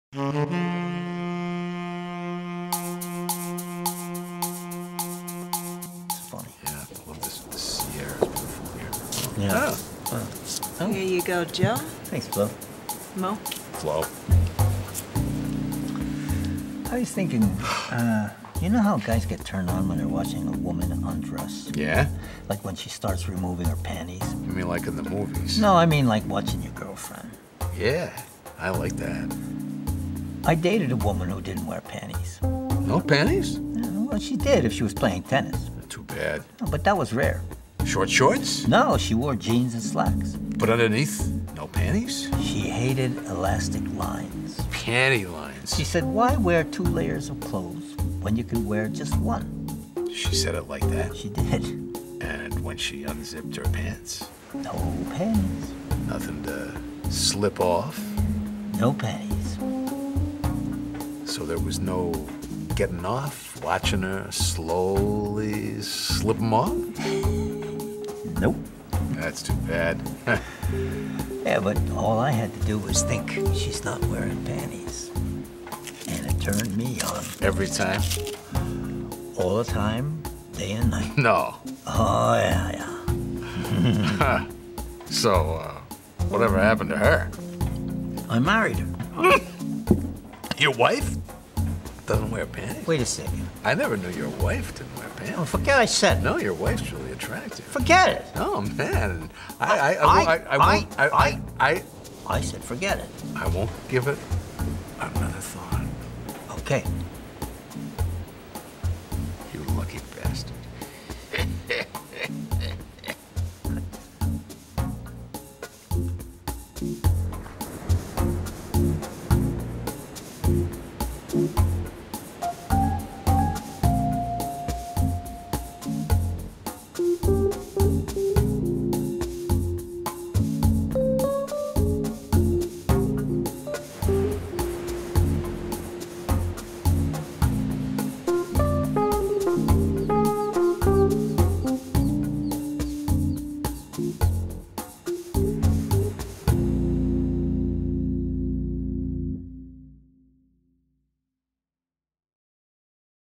4 MINUTE FILM NOIR season II - episode 2 - THE WOMAN who DIDN'T WEAR PANTIES
Film Noir in the tropics?  Think "KEY LARGO".  Anyway, in the Caribbean, Joe regales Moe with a tale of a woman who didn't care to wear a certain undergarment, and this woman is none other than -- well, why spoil it?  Our new cast member Flo makes a brief appearance.  Did I say "brief"?